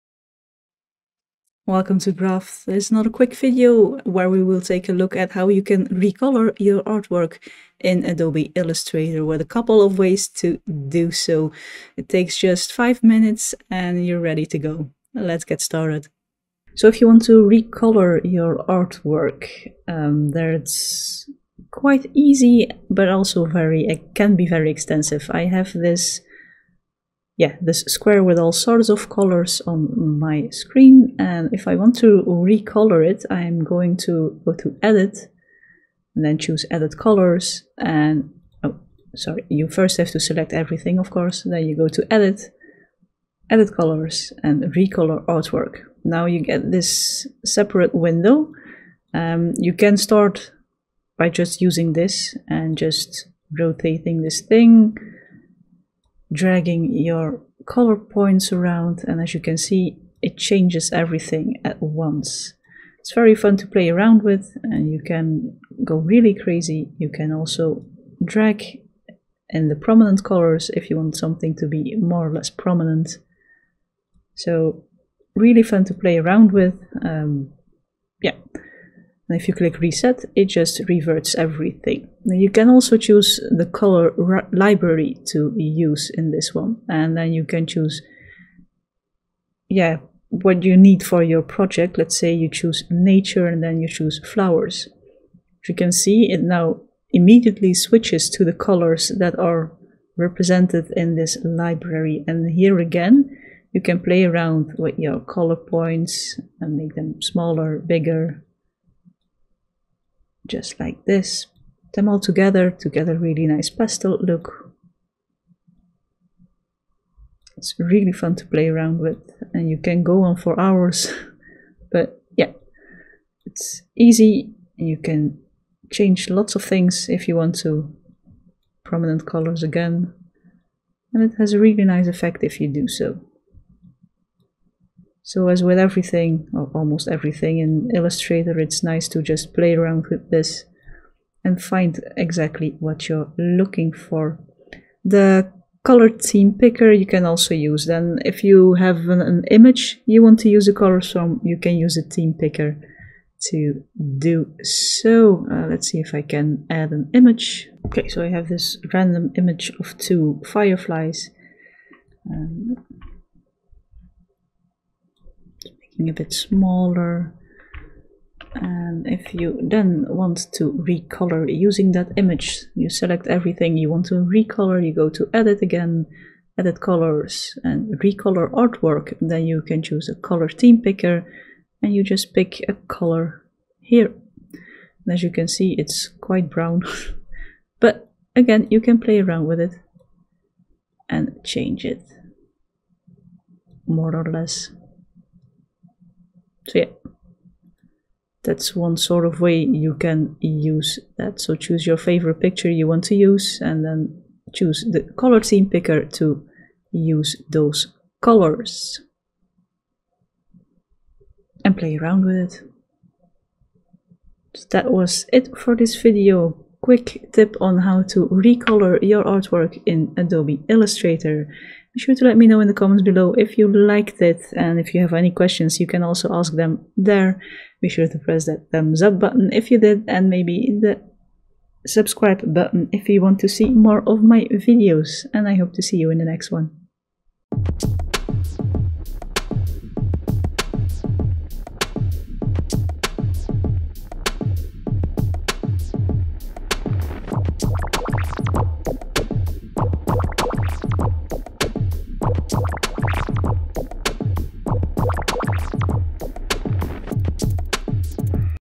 How to: Recolor your artwork - Adobe Illustrator 2022
In this video I will show you different ways to recolor your artwork in adobe illustrator, in just 5 minutes.